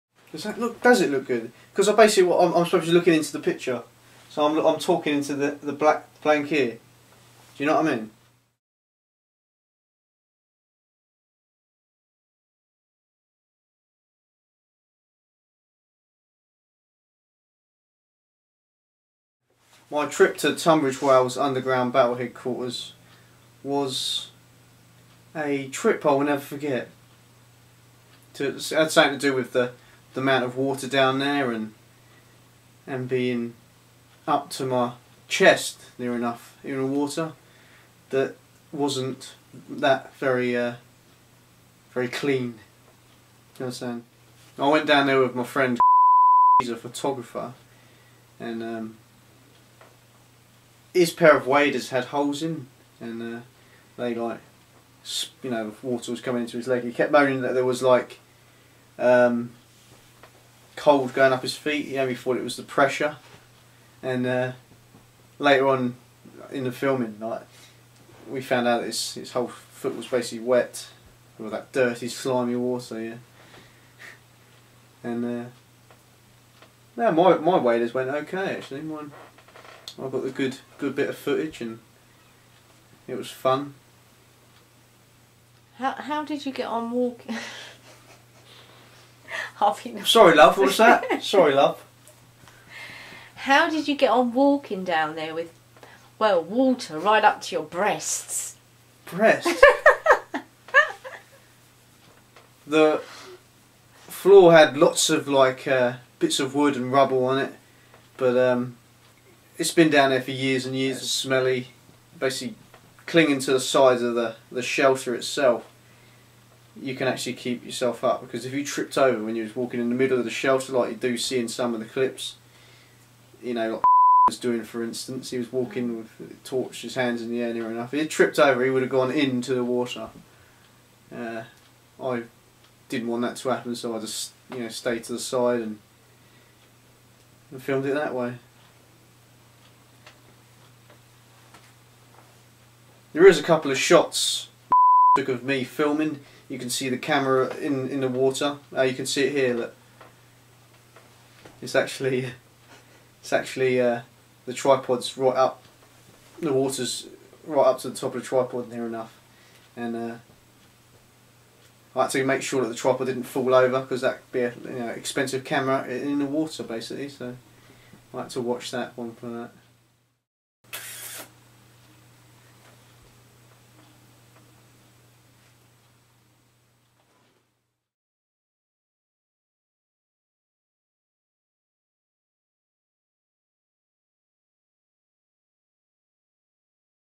Forgotten WW2 Hidden Deep Bunker (Tunbridge Wells) 2008
This was a video i filmed in 2008 to document my journey into the underground WW2 tunnels in Tunbridge Wells Kent.